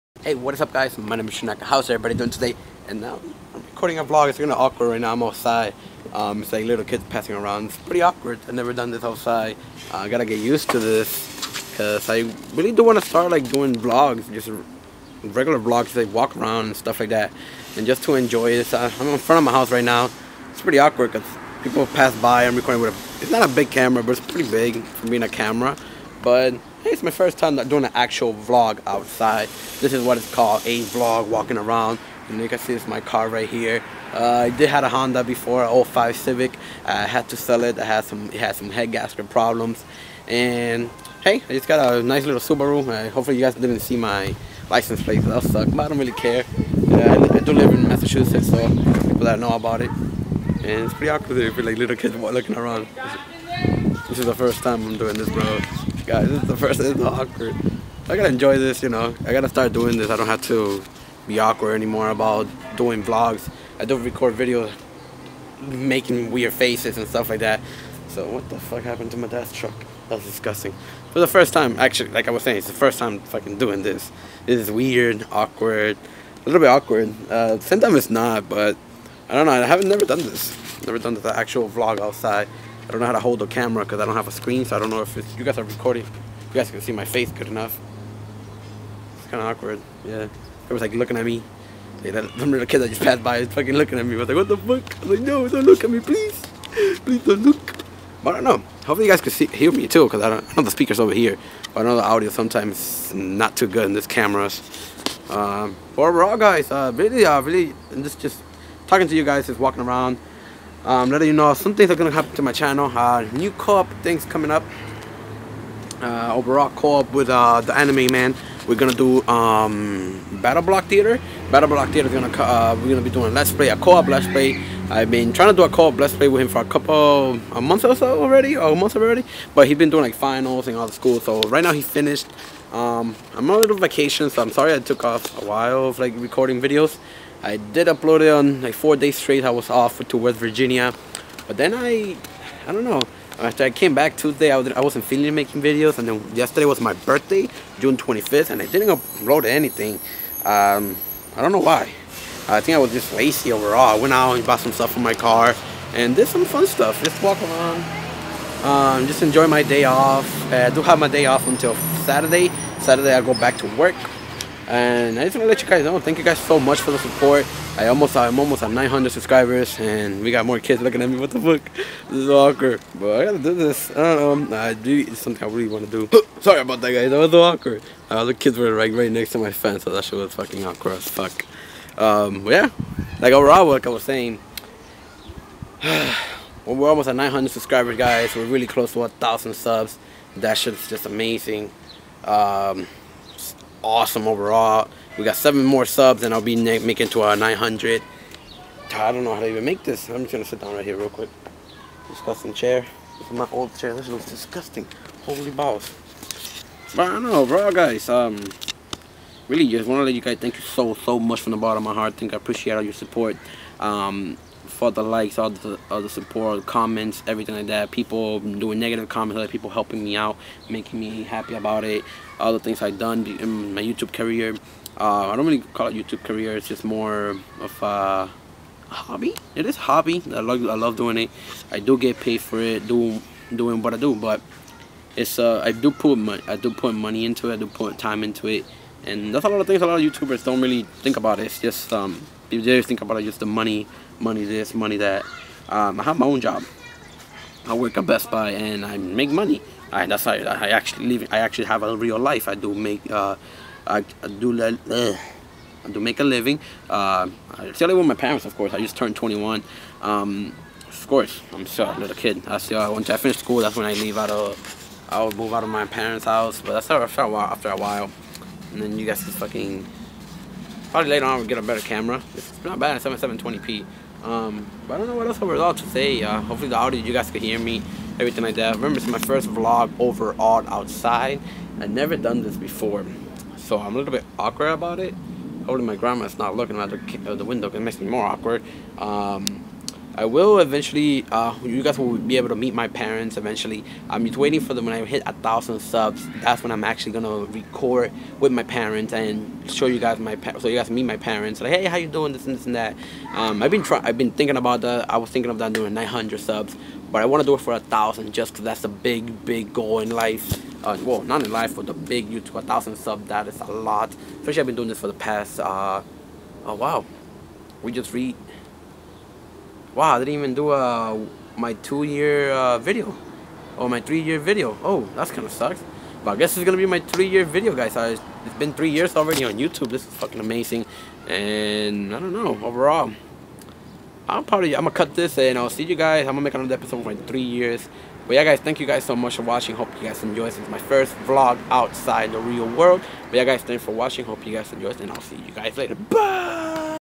FIRST VLOG IN THE REAL WORLD!!!
hahah this is my first  vlog ever outside in the real world kind off awkward. But i have to keep practicing!!! I hope you enjoy this hahah more to come lol!!